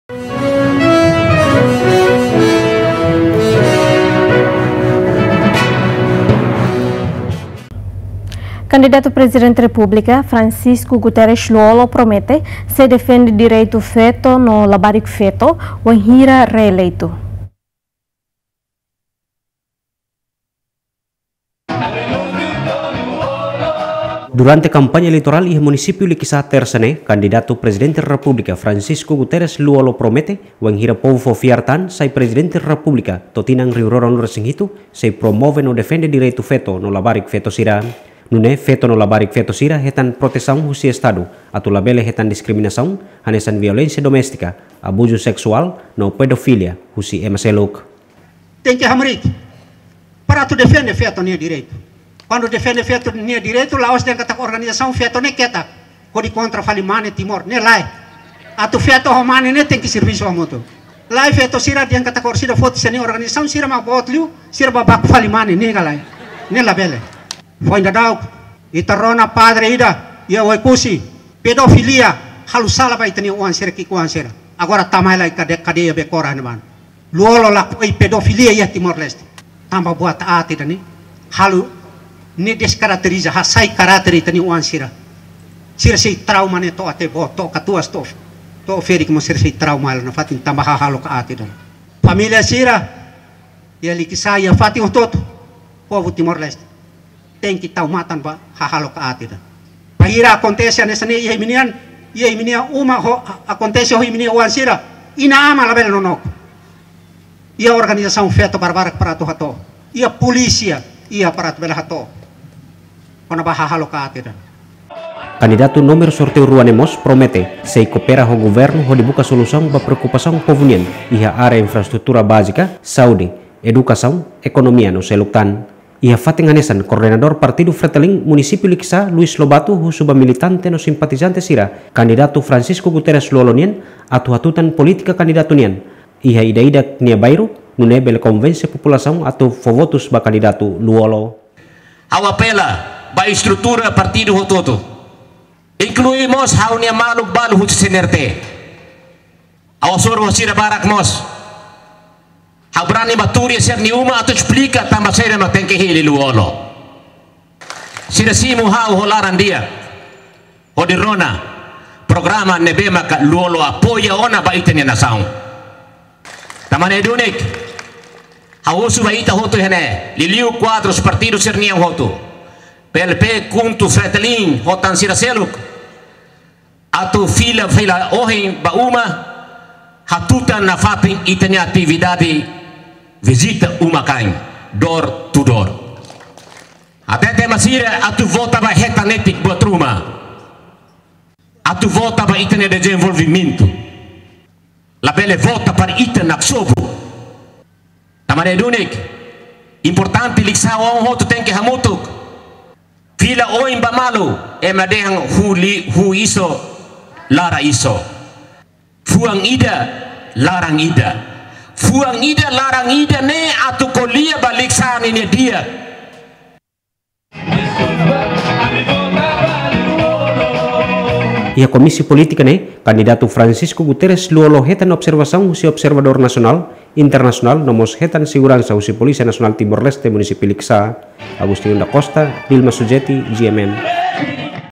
KaPrez Lú Olo lakohi kazu pedofilia akontese tan iha TL
KaPrez Lú Olo sei prioritiza asuntu “feto no labarik feto”, lakohi kazu pedofilia akontese tan iha TL